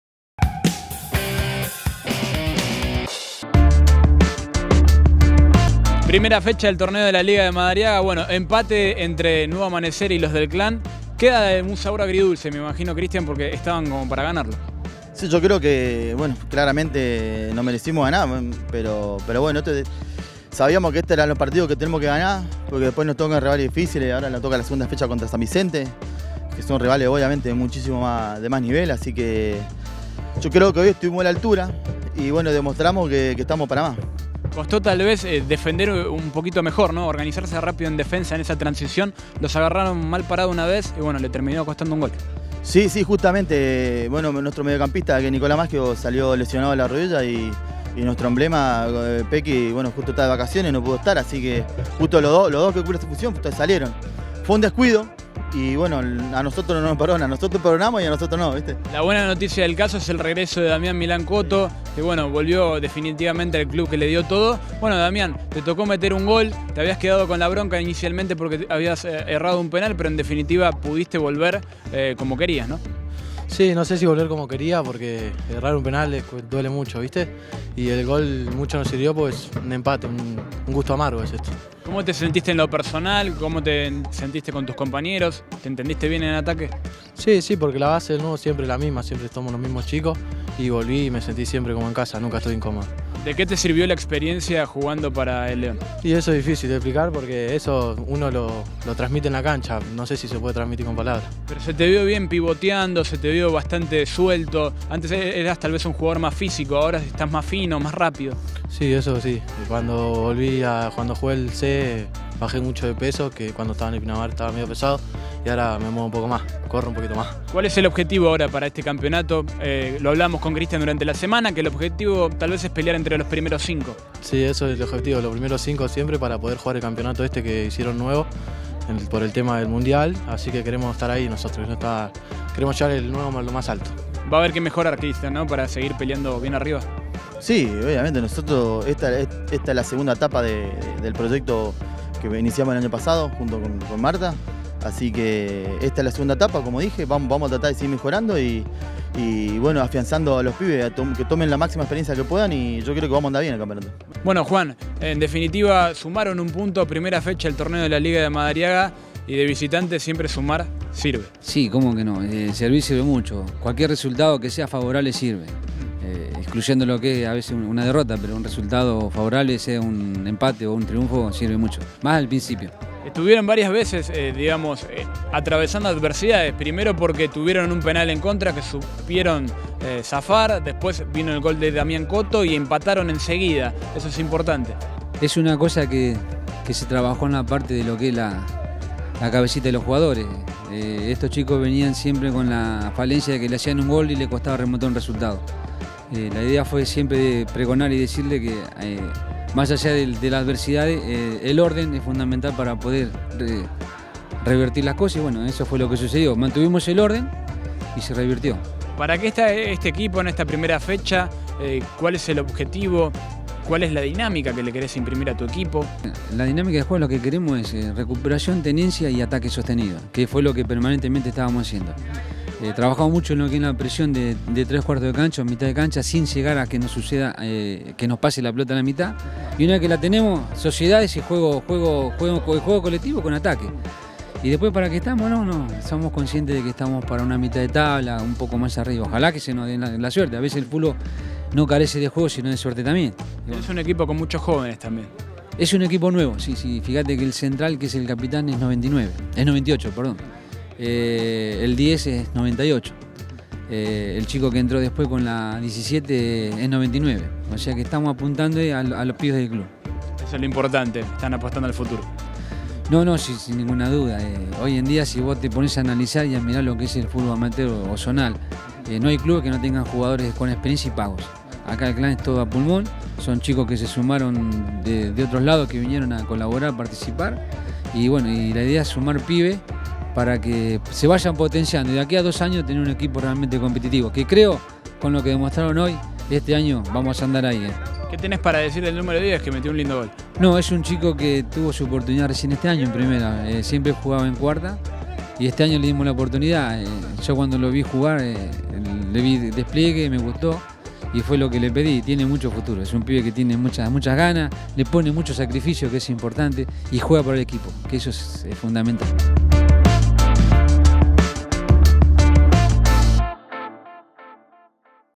NUEVO AMANECER Y LOS DEL CLÀN EMPATARON EN EL DEBUT
Con el regreso del goleador Damián Milán Coto, que erró un penal y convirtiò de cabeza, Nuevo Amanecer empató en el debut. Los Del Clán rescatò un punto como visitante.